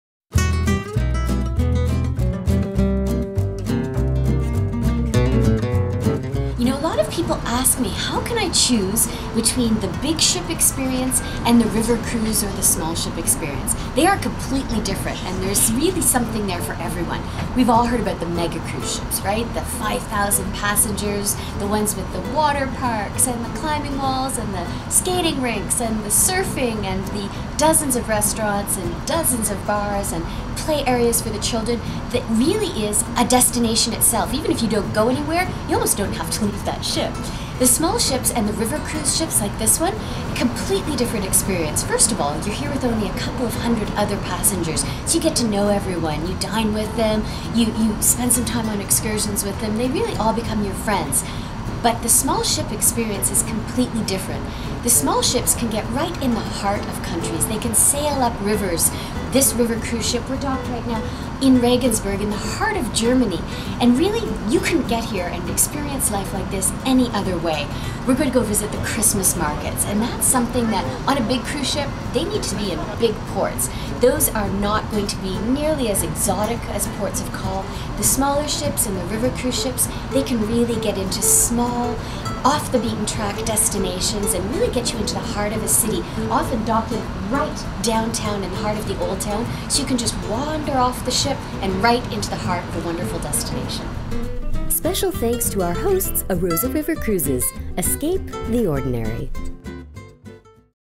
How to decide between a Mega Cruise or a River Cruise?
Can't decide between river cruising or big ship travel? Cruise expert Lynn Elmhirst demystifies the difference.
Travel Video Producer: Garine Tcholakian